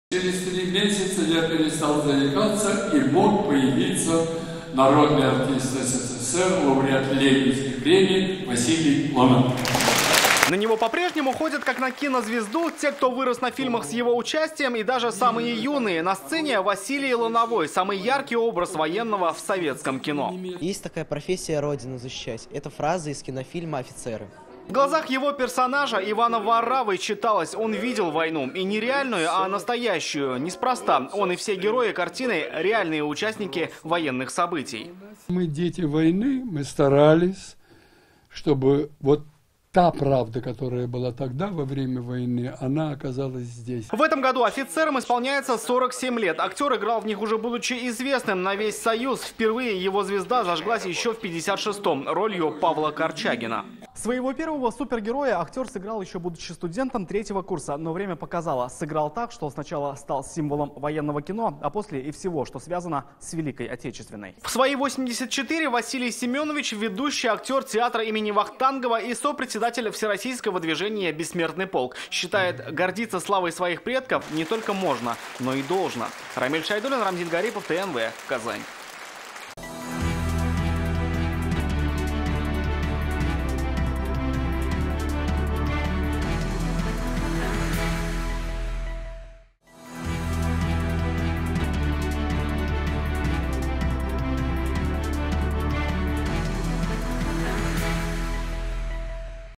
О войне - так, как знает сам - ТНВ
О войне - так, как знает сам. Сегодня с казанцами встретился Василий Лановой. Актёр, с чьим именем связаны самые знаковые советские военные фильмы. Да и сам он - дитя войны. Легко ли ему больше полувека быть героем киноэкрана?

Координаты:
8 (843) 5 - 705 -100